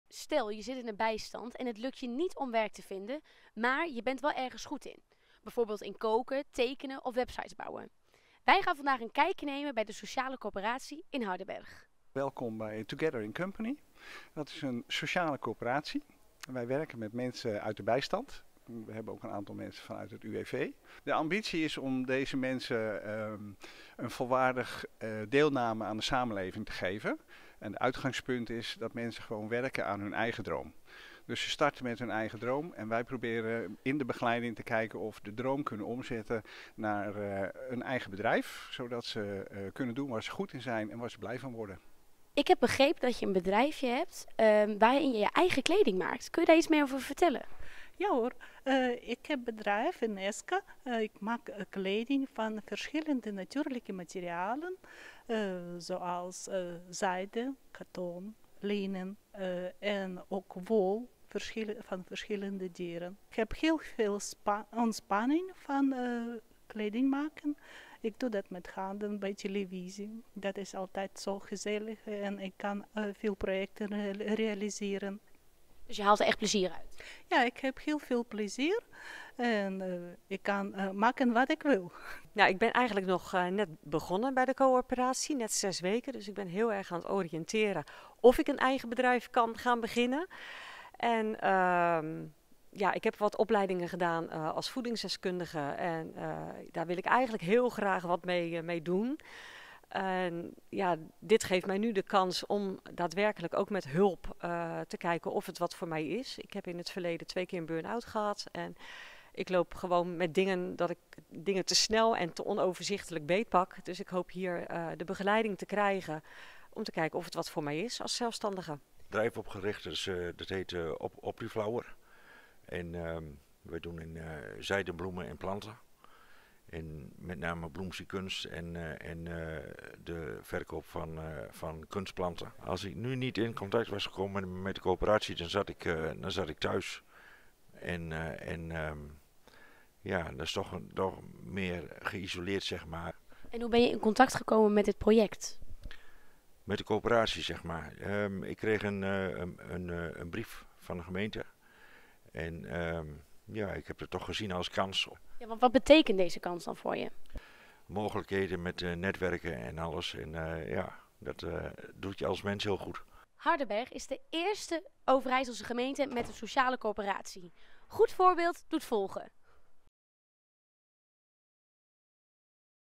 Sociale coöperatie Hardenberg
Elf ondernemende Hardenbergers met een uitkering begonnen in januari met het opzetten van een eigen bedrijf, zonder dat ze gevaar liepen hun recht op bijstand te verliezen. Samen vormen ze de sociale coöperatie Together in Company, waarin ze elkaar ondersteunen en stimuleren. Negen maanden later is al een handvol start-ups van de grond gekomen.